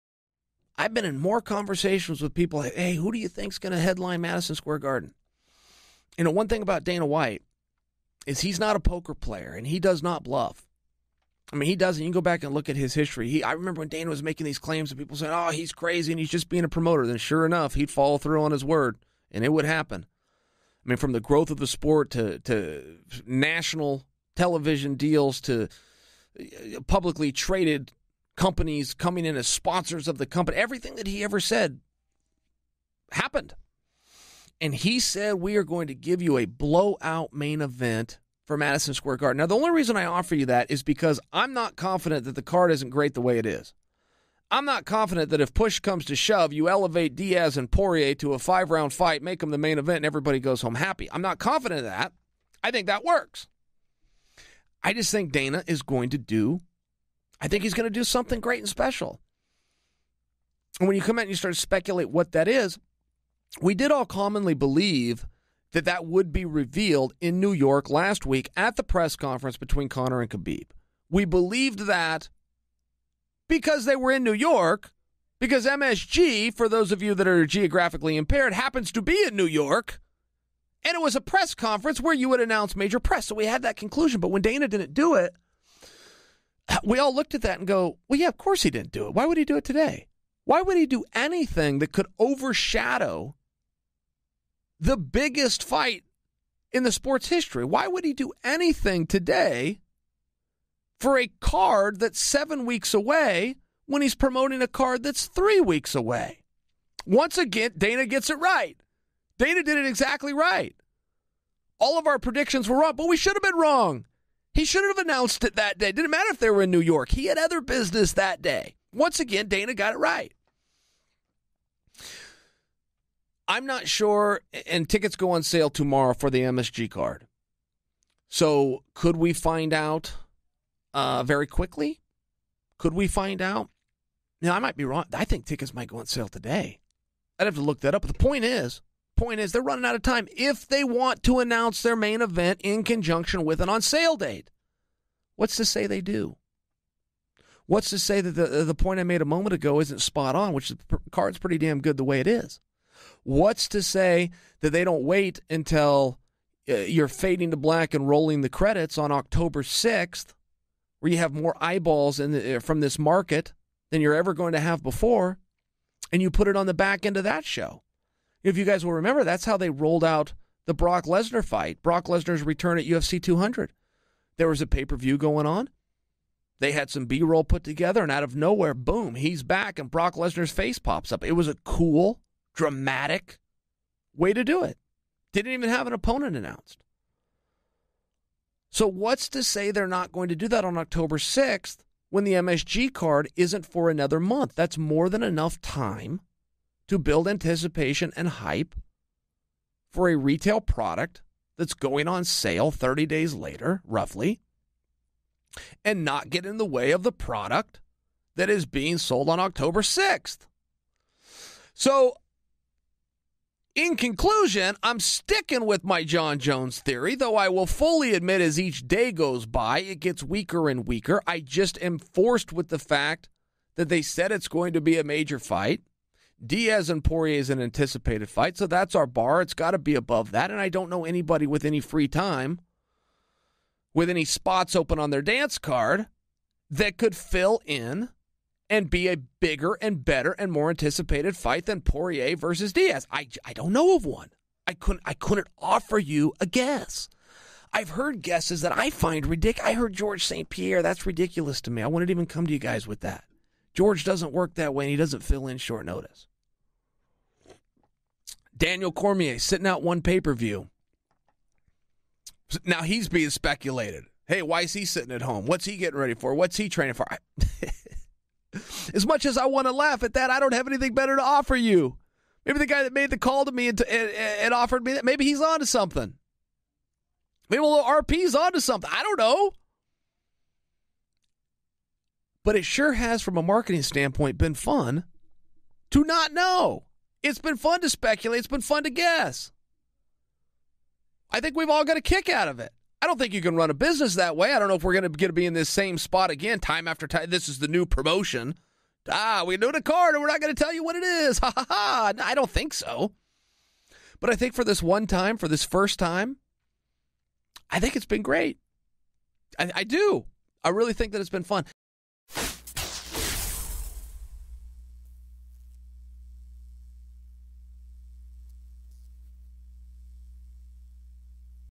UFC Mystery Main Event Marketing - Dana White Announcement
At this moment there is still no main event for the UFC Madison Square Garden event in November. Although this is not a common way to market a UFC card...is it working?